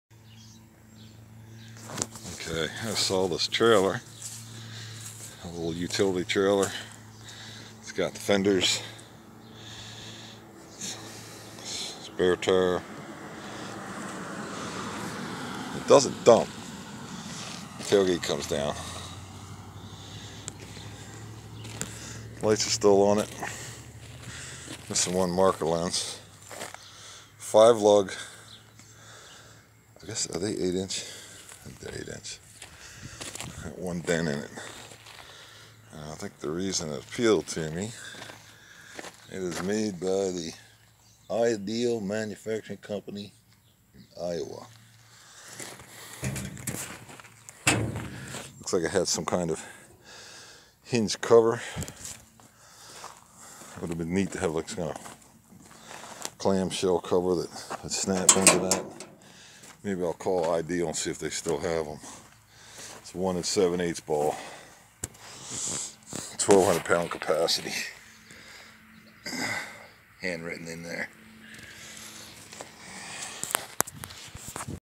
Small utility trailer made by ideal
44" x 56" bed on 4.80 x 8" tire with 1 7/8" ball